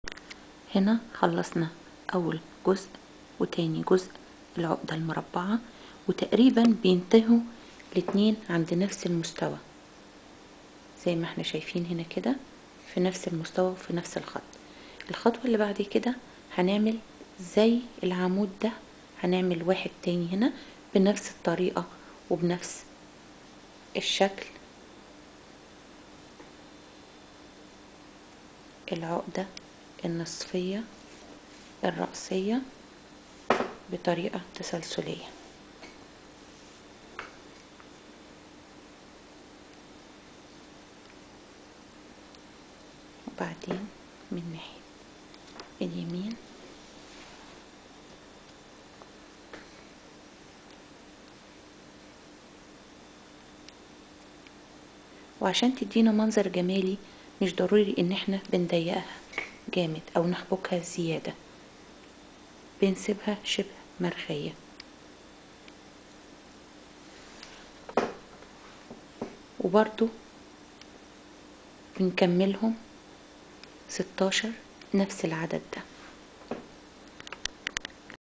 6- تعليقة المكرمية 6 - macrame Wall ornament
طريقة عمل العمود الثاني في اول مستطيل  بتعليقة المكرمية